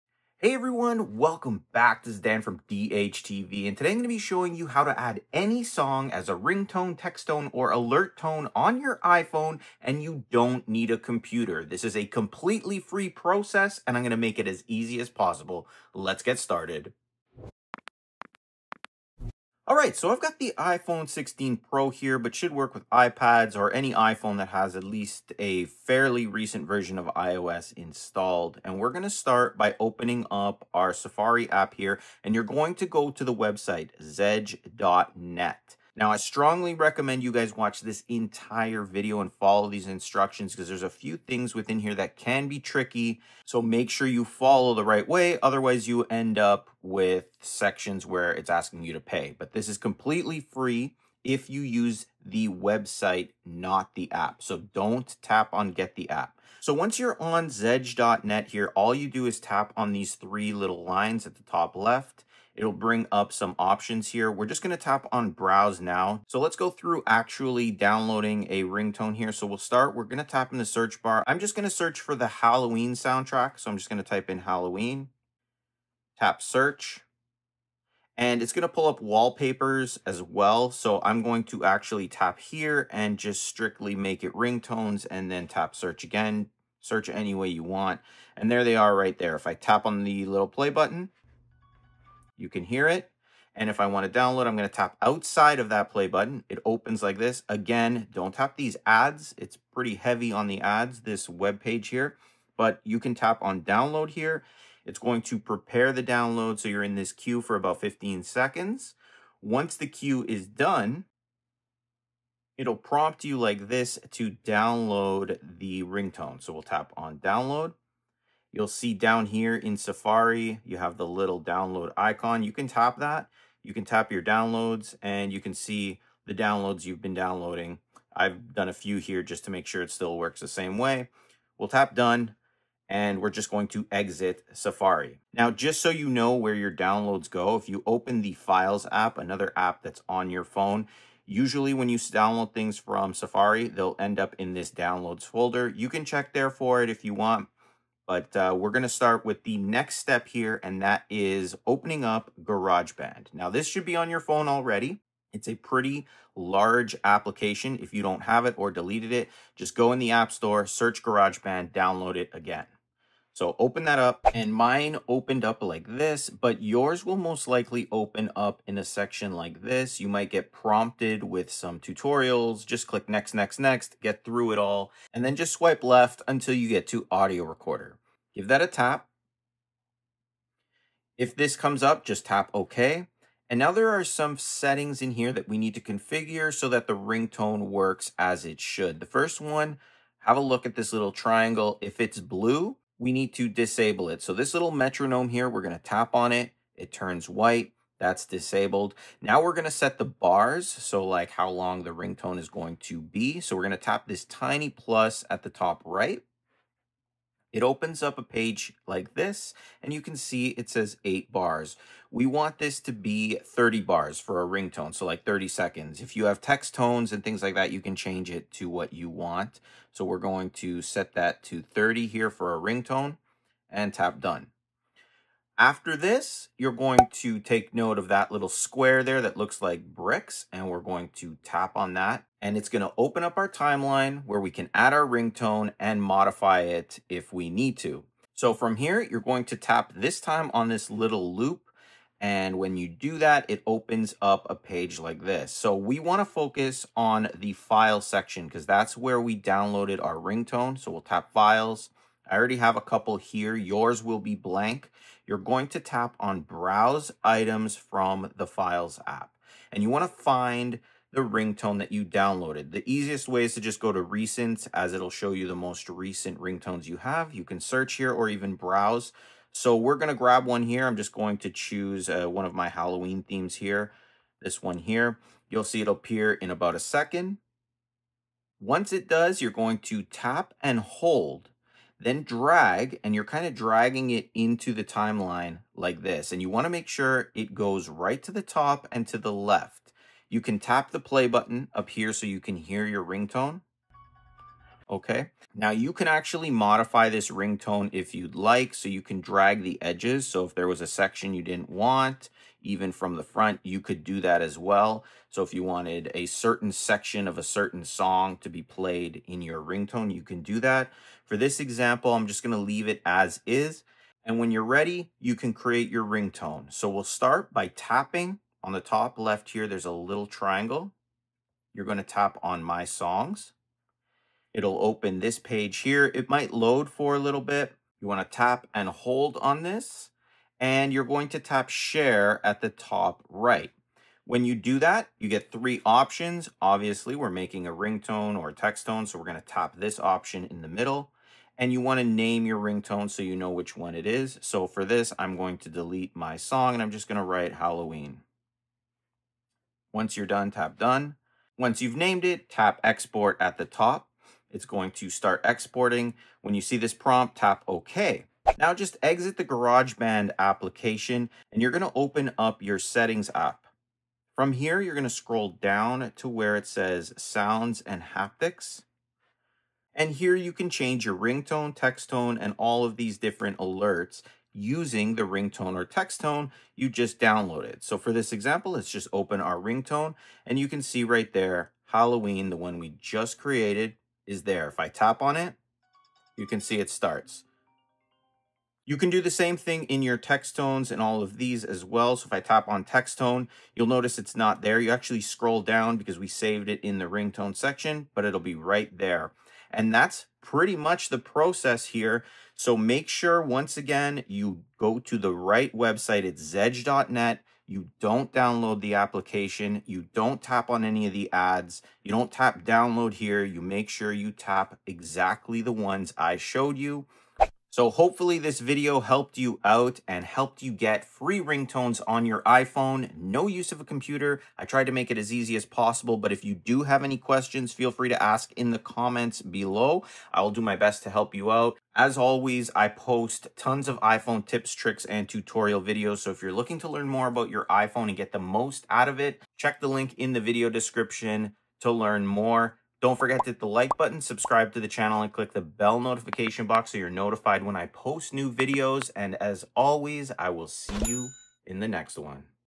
Stop Paying For Ringtones! - How To Get Free Ringtones On iPhone (No Computer)
In todays video, I’ll show you how to get free ringtones on iPhone without a computer and set any song as an iPhone ringtone in 2025/2026. This works on pretty much any iOS 18 device — whether you're using the iPhone 16 Pro Max, iPhone 15, iPhone 14, iPhone or even an iPad Pro.

This is a completely free method that does not require a computer, iTunes, or paid apps. You’ll also be able to set songs and tones as text tones, alerts, or notification sounds using this method.

We’ll be using Zedge website to get free iPhone ringtones, then I’ll walk you through how to use GarageBand to convert and apply the ringtone, and finally how to set it up in the Sounds & Haptics section of the Settings app.

Chapters: How To Install Free Ringtones For iPhone
00:00 Intro to the Ring Tones Tutorial
00:28 How to Download iPhone Ringtones Free
02:28 How To Turn Music Into A Ringtone
05:58 How To Change iPhone Ringtone

If you ever wanted to customize your iPhone ringer or iMessage sound, this is the easiest way to do it!